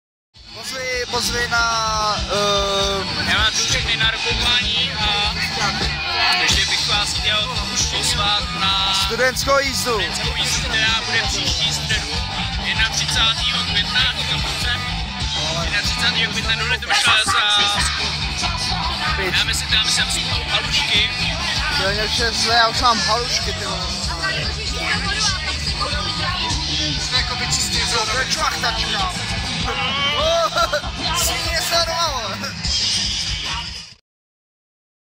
DIRTY WAY - Pozvánka na Studentskou jízdu
Kapela DIRTY WAY, přesněji Wicky, si vás dovoluje pozvat na náš nejbližší koncert :)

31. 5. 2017 - LITOMYŠL / Studentská jízda